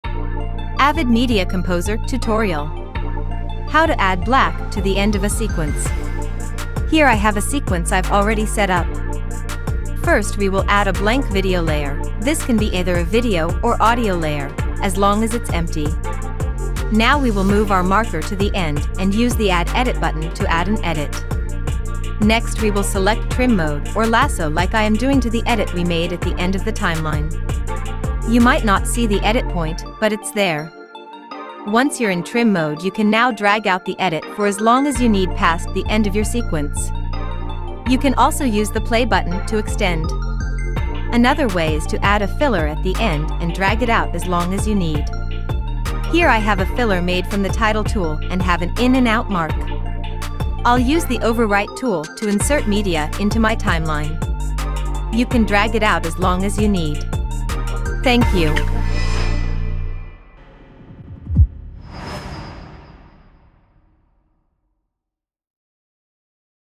AVID MEDIA COMPOSER TUTORIALS HOW TO ADD BLACK TO THE END OF A SEQUENCE
Welcome to our Avid Media Composer tutorial series! In this episode, we'll show you a simple yet crucial technique that will elevate the professionalism of your videos. Learn how to add black at the end of a sequence or timeline in Avid Media Composer.

Whether you're a beginner or an experienced editor, adding black at the end of your videos is a must-know skill.

In this step-by-step tutorial, our expert editor will guide you through the process, sharing insightful tips and tricks along the way. Discover the various ways to add black to your sequences or timelines, including adjusting the duration with the add edit button or using the default black clip created in title tool.

By the end of this tutorial, you'll have the knowledge and confidence to add black to end of your sequence in your Avid Media Composer projects effortlessly.